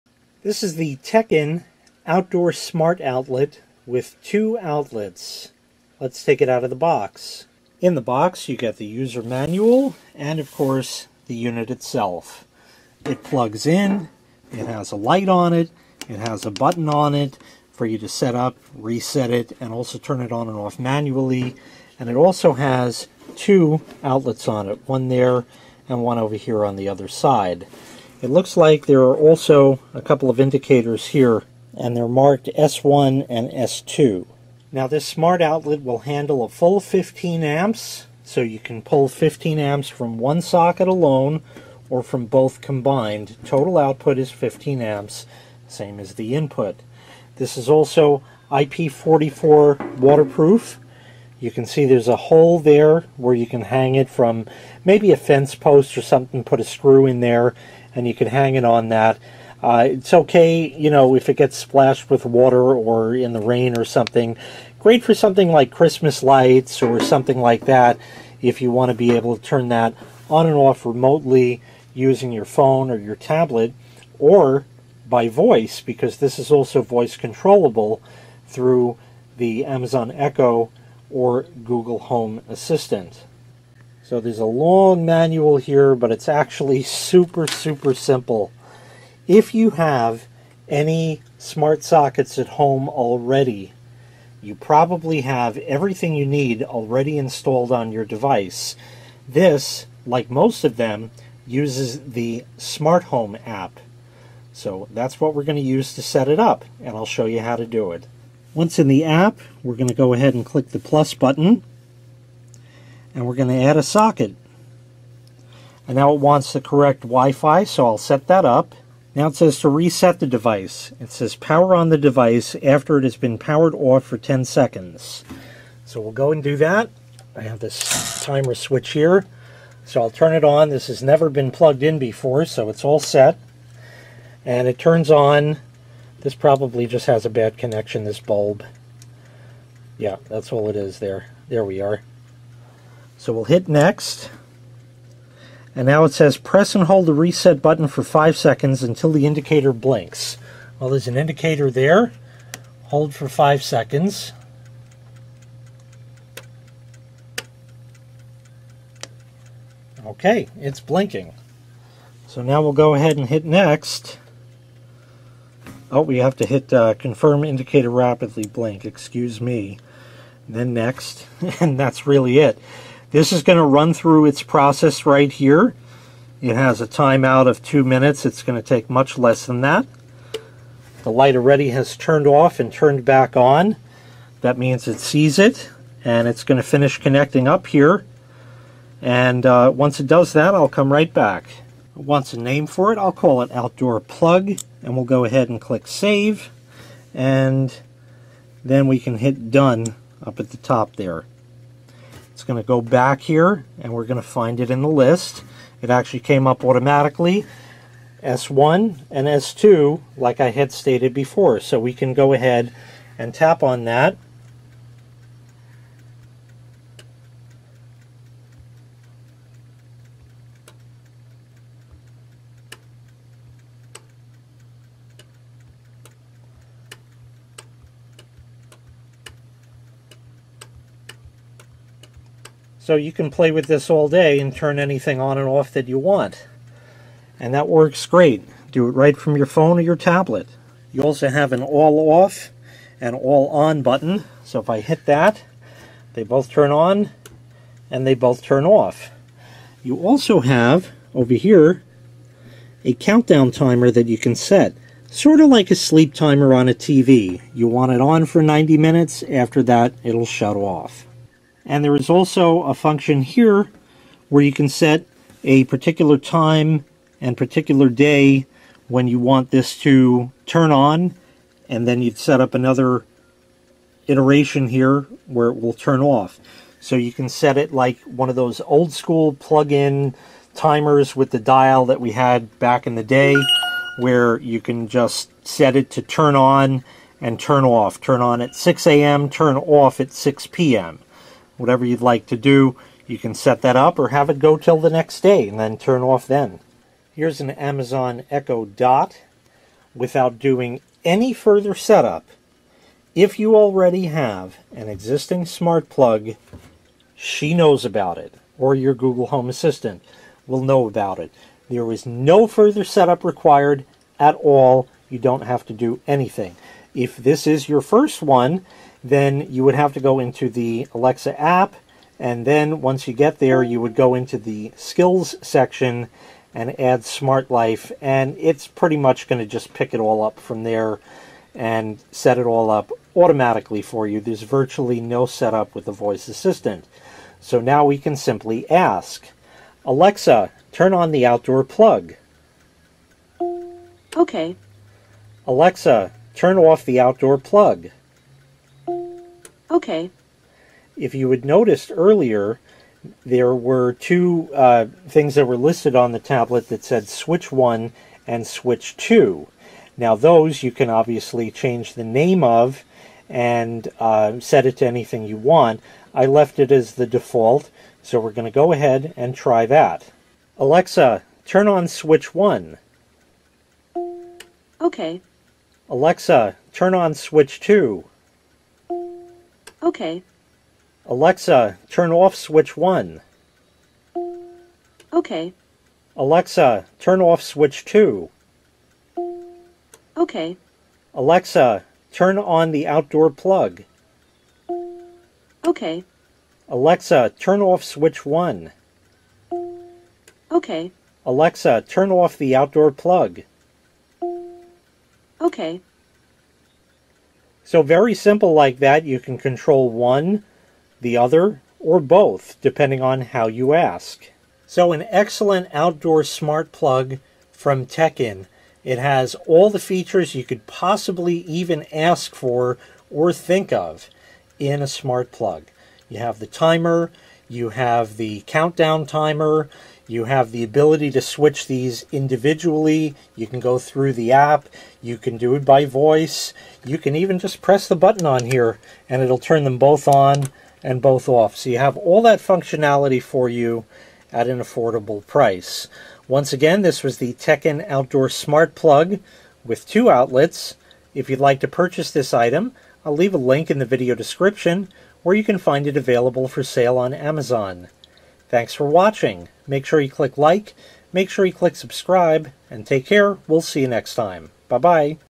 TECKIN 2-Outlet Outdoor Smart Plug Review
This is the wi-fi outlet we've all been waiting for!  It is an outdoor-rated wi-fi outlet.  Of course you'll need to make sure that you have adequate wi-fi coverage outside, which can easily be tested with your phone.

It's got 2 outlets on it, and acts as 2 wi-fi outlets in one.  You do get a full 15 amps through it, but being that it's split 2 ways, you cannot exceed 15 amps total through it.  (It's not split 7.5-7.5, you can slice and dice it any way you want).

It also is voice controlled, using either Amazon Echo or Google Home.  Setup is very easy, works just like any other wi-fi outlet.  Very easy, and the app allows you to individually control each outlet on it.  Great for holiday lights--imagine the luxury of asking your voice assistant to turn your lights on!